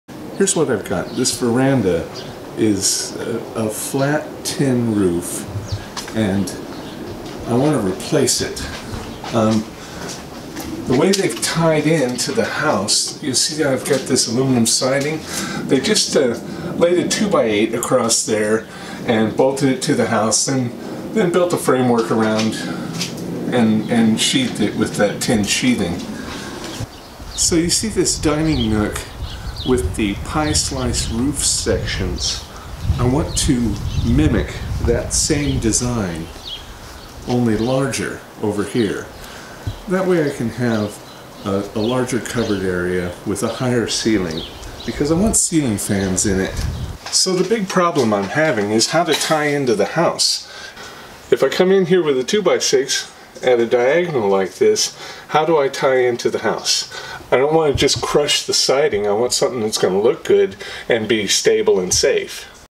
Wildman Veranda House Tyin
Here I show my current veranda and explain the new veranda I want to build.